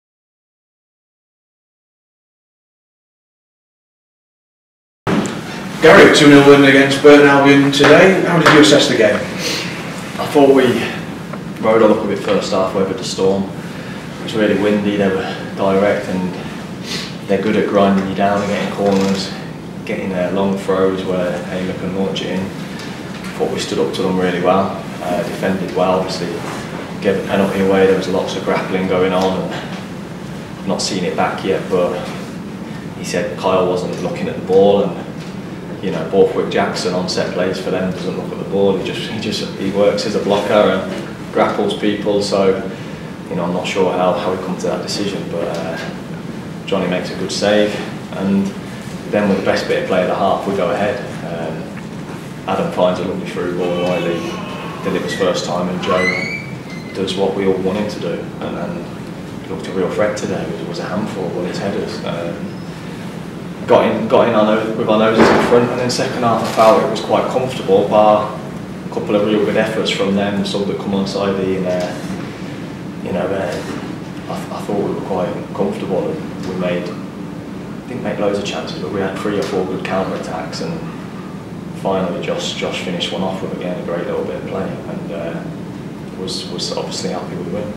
Gary McSheffrey on beating Burton Albion
Gary McSheffrey spoke to the media following the 2-0 win over Burton Albion in Sky Bet League One.